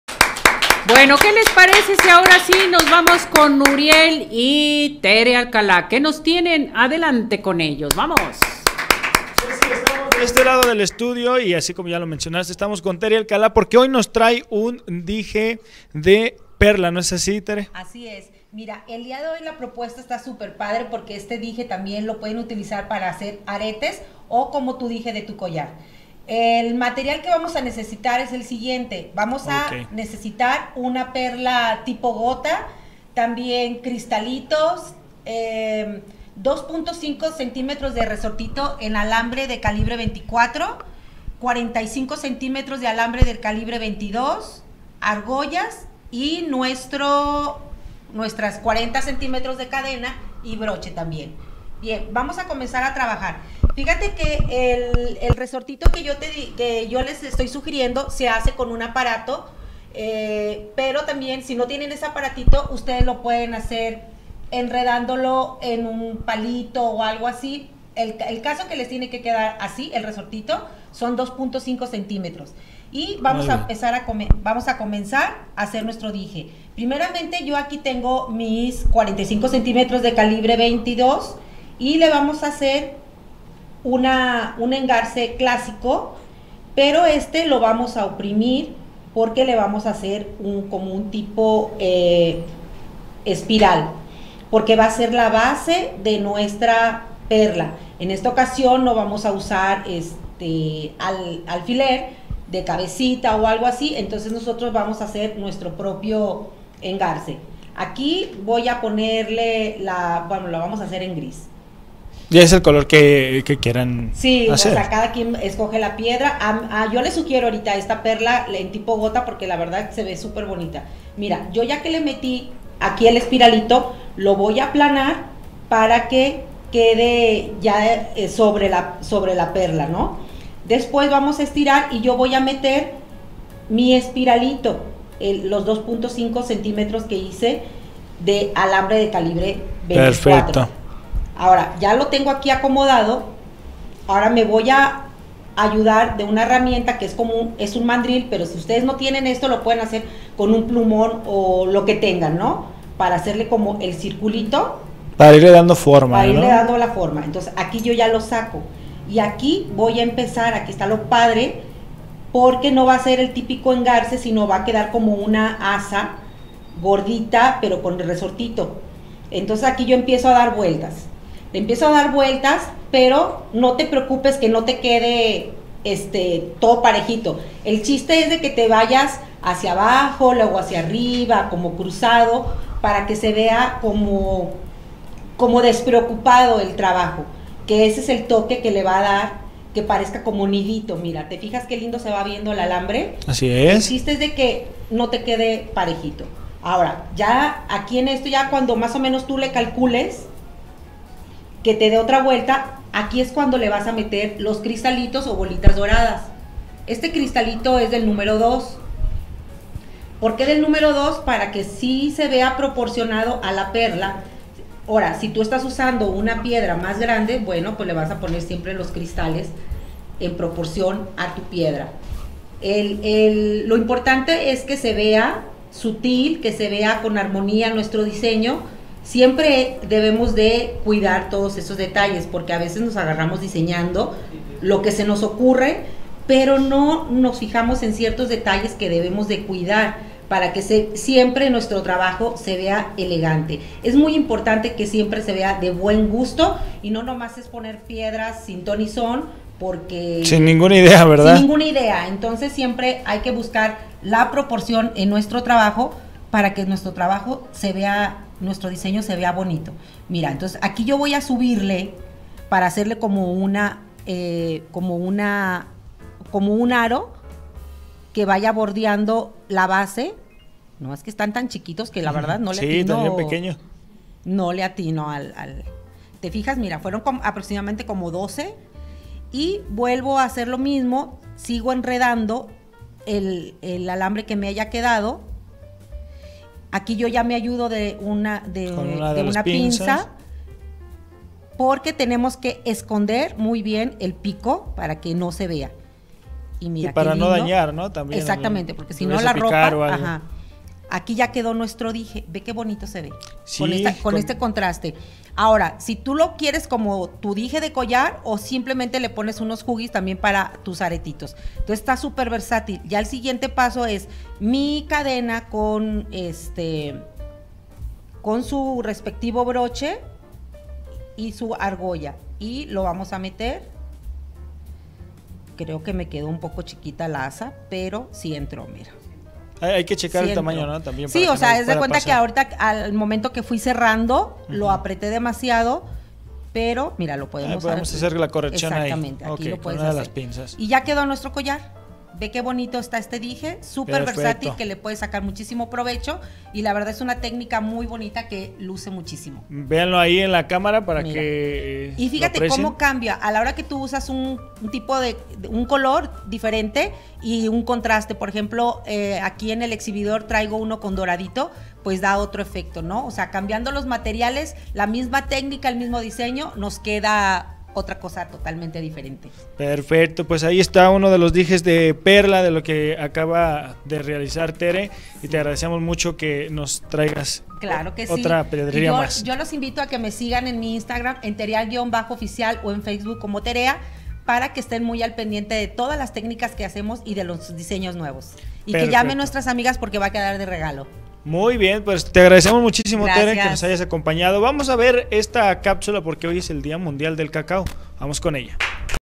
Tutorial: Collar con dije de PERLA📿 | DIY Joyería | Accesorios paso a paso
¡Luce increíble con un hermoso detalle! Crea un collar de tendencia con dije de perla en alambrismo, con Tere Alcalá, experta en diseño y armado de joyería.

Necesitas estos materiales para realizarlo
* Perla gota
* 2.5cm de espiral alambre #24
* 45cm alambre #22
* Cristalitos
* 40cm cadena
* Broche
* Argollas